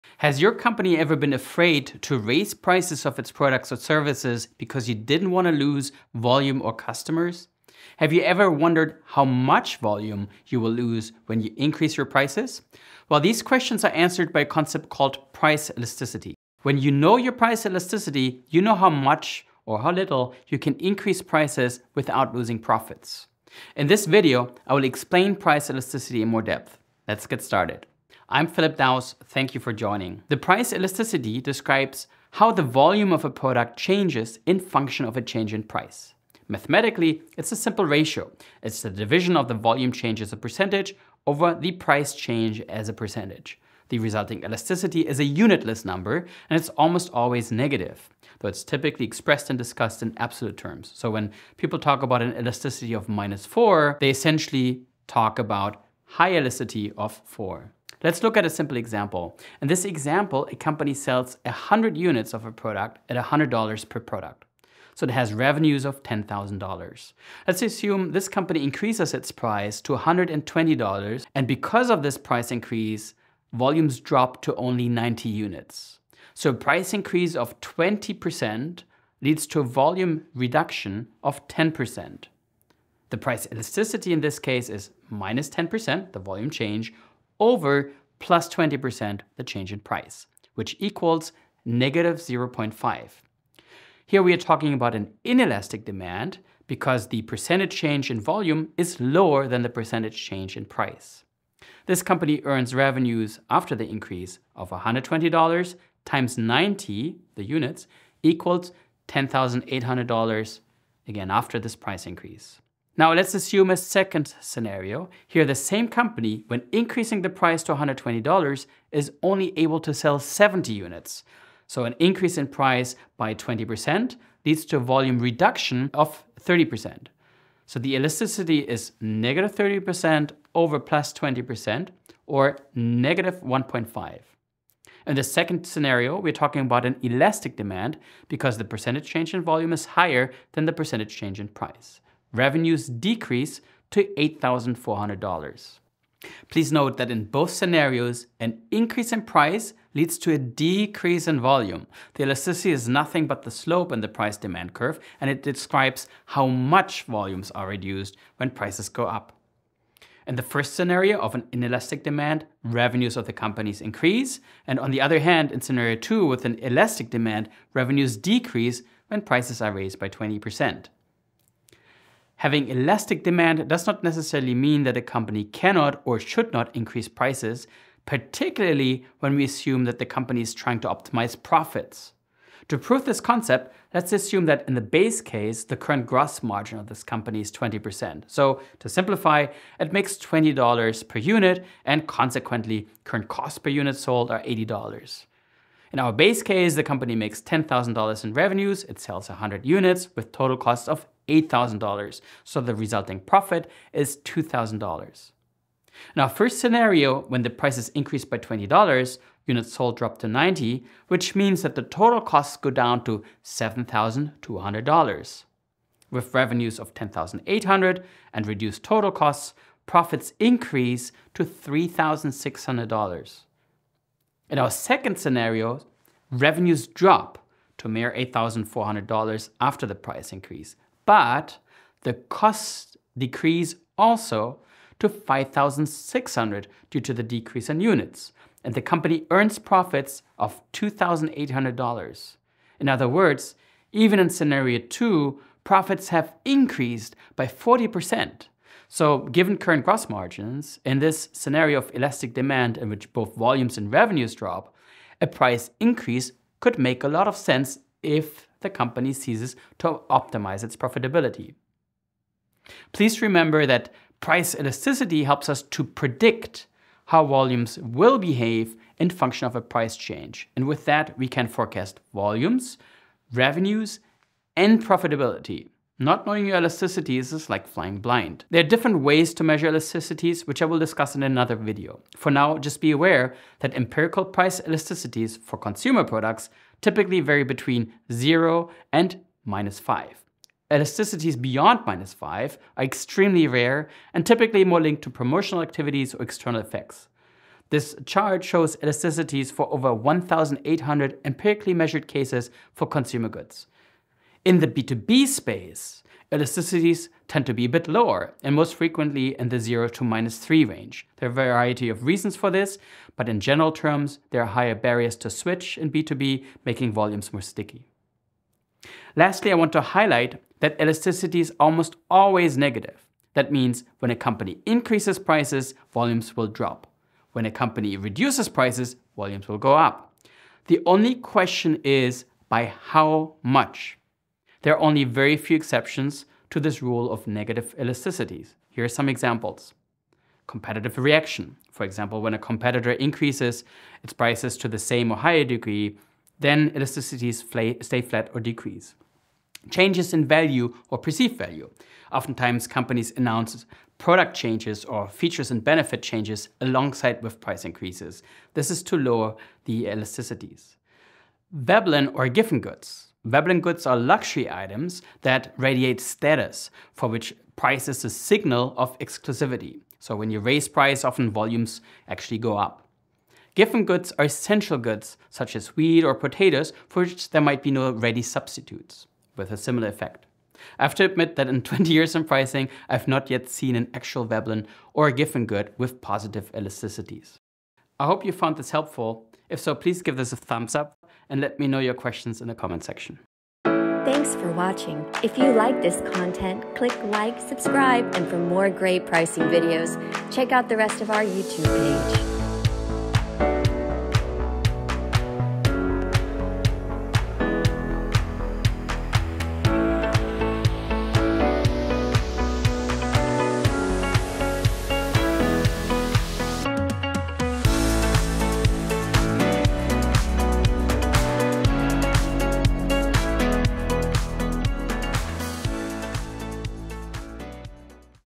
Price Elasticity Introduction and Simple Explanation
In this video you'll get the easiest and simplest introduction to Price Elasticity that you will find on YouTube. Price Elasticity is extremely useful as you can predict how your volumes and profits will react when there is a price change. Not knowing your price elasticity is like flying blind.

Here is my essential follow up video on how to measure price elasticity using 6 different sources with methods

Use the chapters below to get the info you need quickly:
Introduction 00:00
Definition 00:28
*Mathematic Equation for Price Elasticity 00:35
Inelastic Demand 00:59
Elastic Demand 01:52
Comparing Inelastic & Elastic Demand 02:24
Price Optimization with an Elastic Demand 02:51
What Price Elasticity helps Predict 04:23
B2C Price Elasticities 04:42
B2B Price Elasticities 05:03
Rule of Negative Elasticity 05:18
Exceptions to Rule of Negative Elasticity 05:34

*Please note that in the mathematic equation for Price Elasticities, I use "V" to represent Volume, however many textbooks will use "Q" to represent quantity of the product.

Look out for my follow up video on how you to measure Price Elasticities!